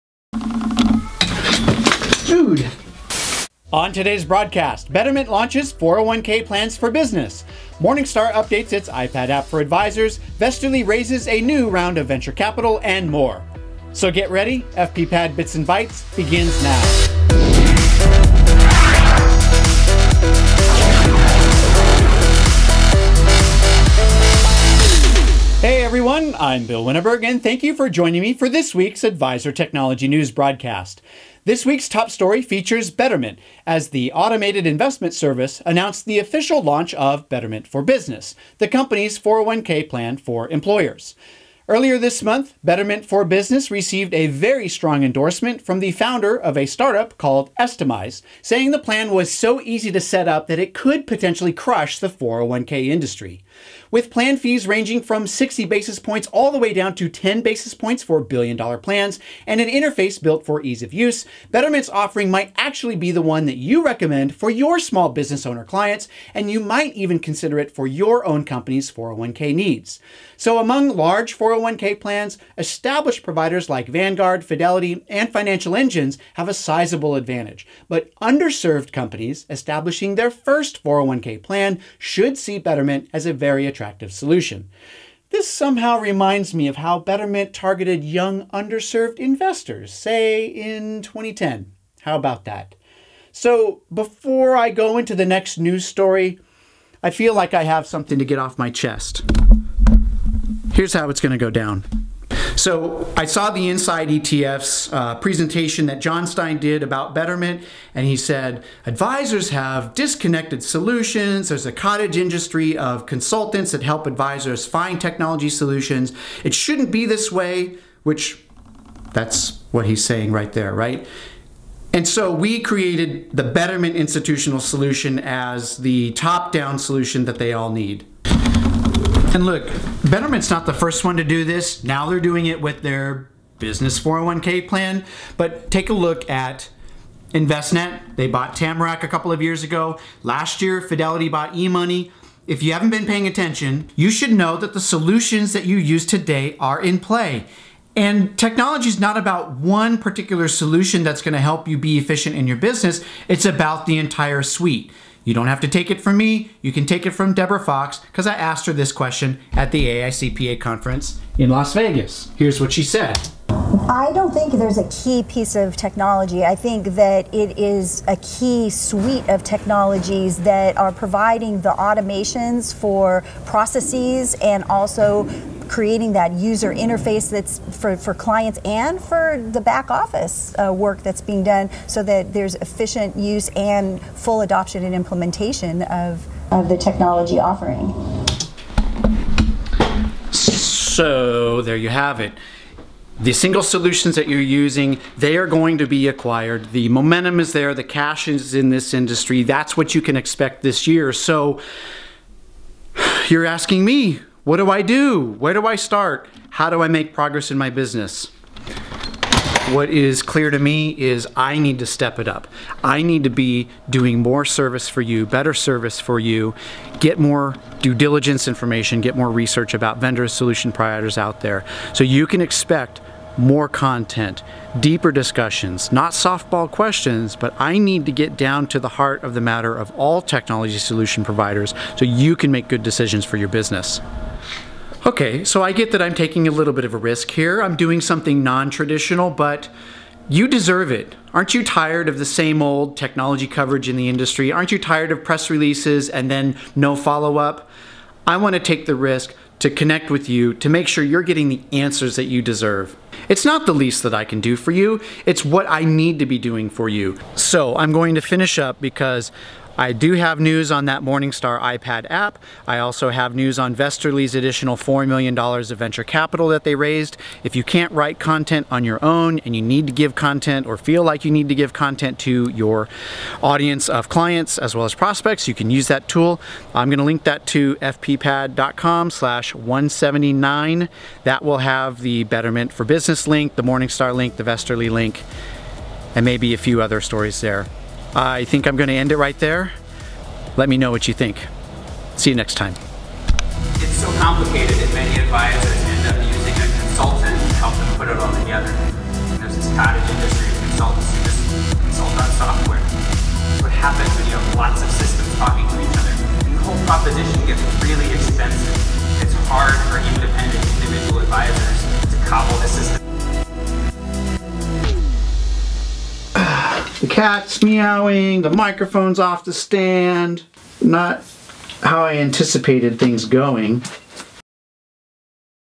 Betterment for Business launches, and you won't believe what happens next
for the links to this week's top stories.

Watch 30 seconds of Jon Stein's presentation at the Inside ETFs conference

So get ready, FPPad Bits and Bytes begins now!